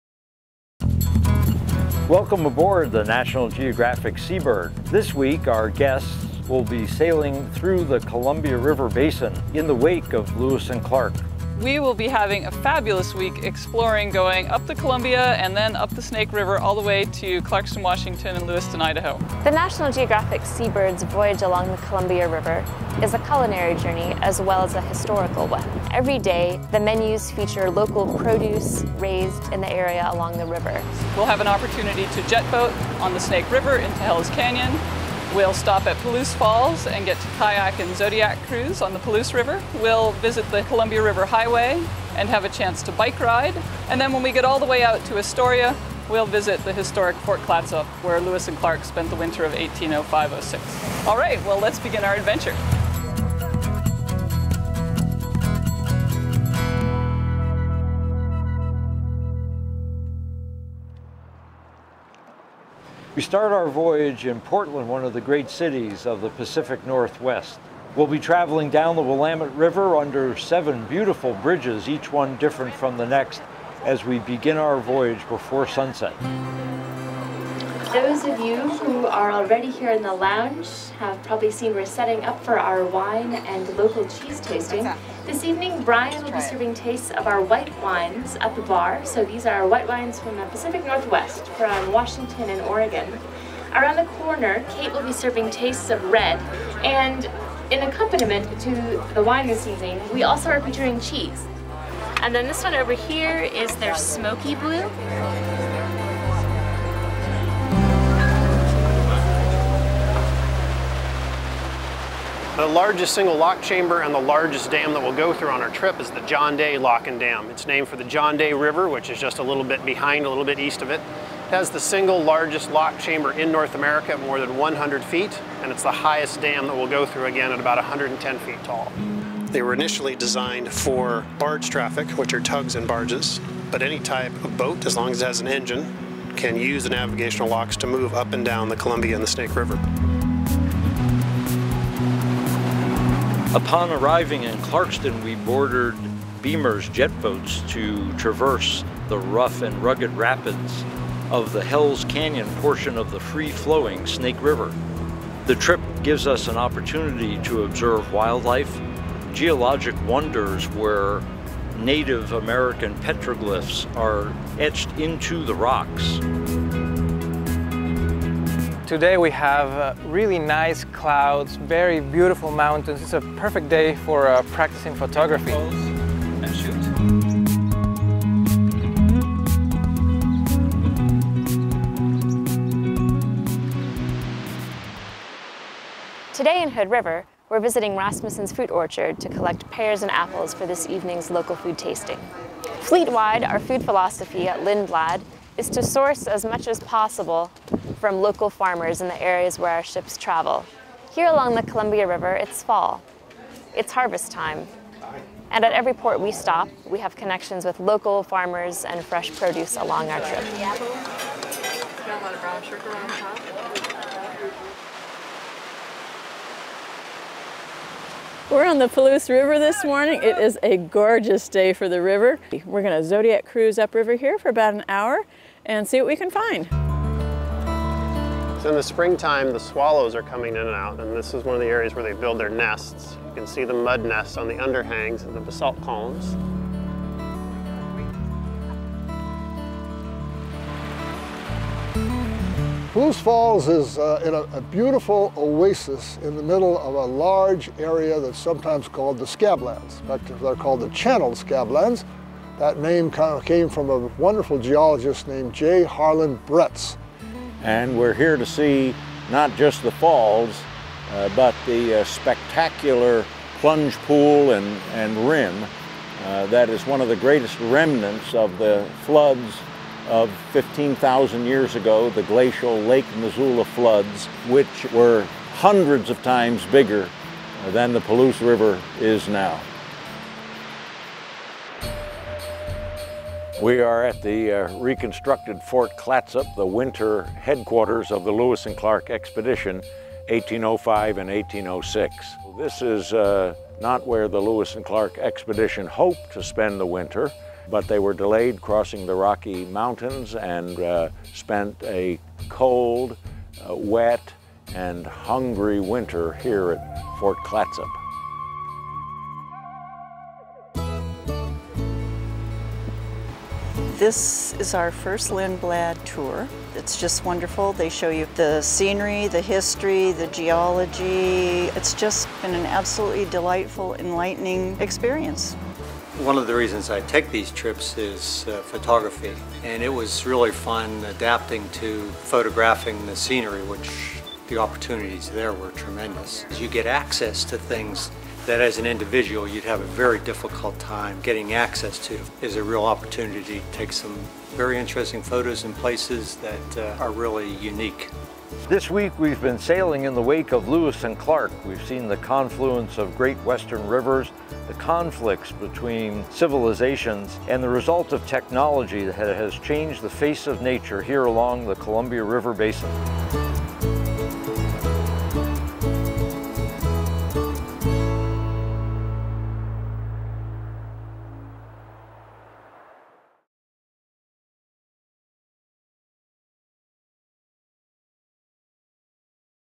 Road Scholar Columbia and Snake Rivers: Journey of Lewis and Clark
Nestled amid breathtaking mountains and striking canyons, the Columbia River Gorge and Snake River region are not only feasts for the natural senses, but are home to a wealth of U.S. history. Experience the best of the region as you stop at local vineyards, learn about the adventures of Lewis and Clark and early pioneers, and marvel at the beauty of Multnomah Falls.